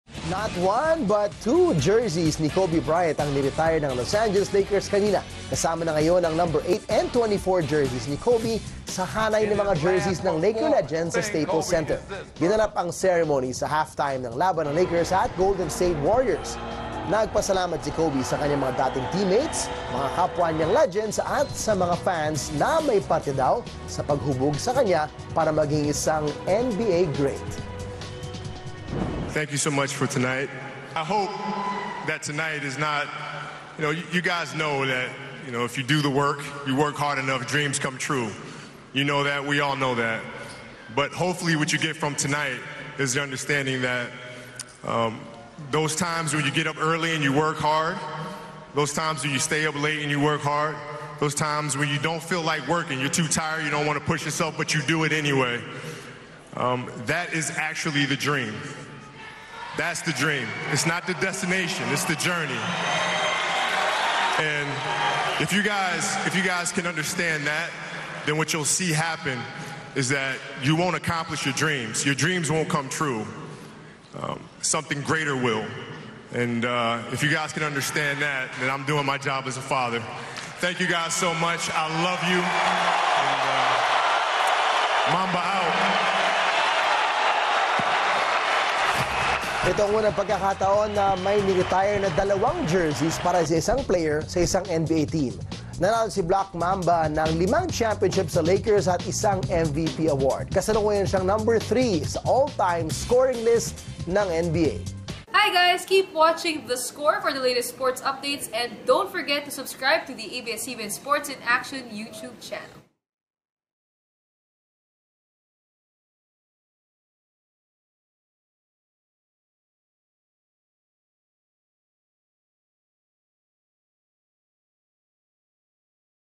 The Score: Kobe Bryant retires two of his Jerseys
Los Angeles Lakers’ Legend, Kober Bryant retires two of his Jerseys. The retirement ceremony was held at the Staples Center during the halftime break of Lakers vs Golden State.